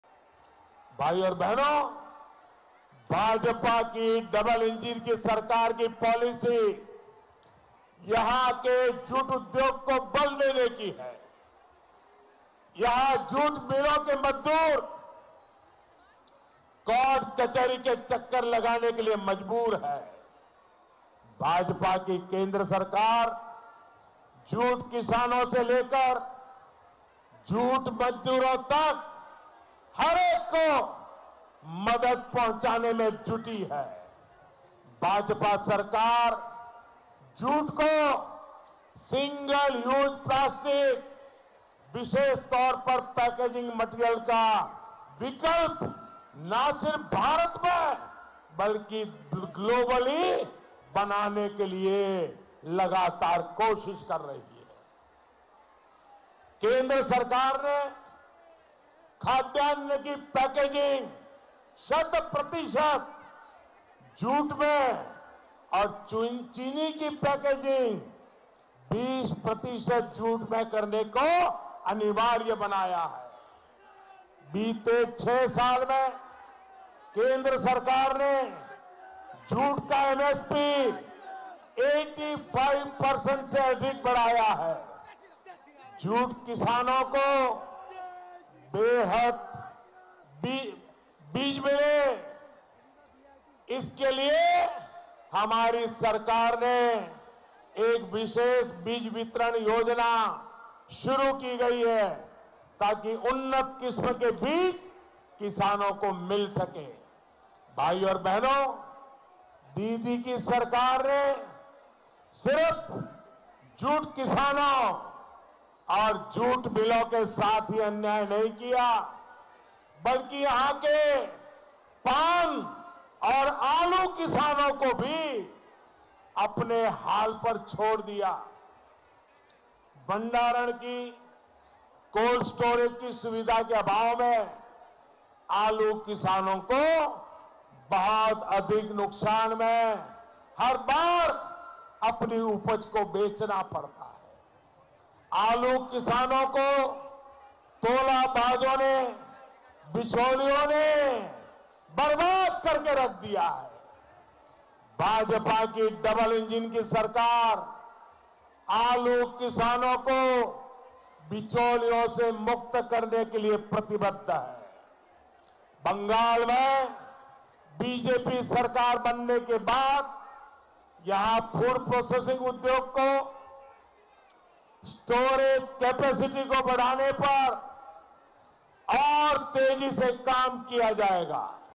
BJP will work for welfare of farmers in West Bengal: PM Modi
PM Modi addressed public meetings in West Bengal’s Jaynagar and Uluberia today. Speaking at Jaynagar, Prime Minister Narendra Modi said, “I can witness the wave of ‘Ashol Poribortan’ being sped up by this region. In the record-breaking turnout in the first phase, people have given massive support to the BJP. PM Modi also paid tribute to late Shova Majumdar.

Prime Minister Narendra Modi addresses public meeting at Uluberia, West Bengal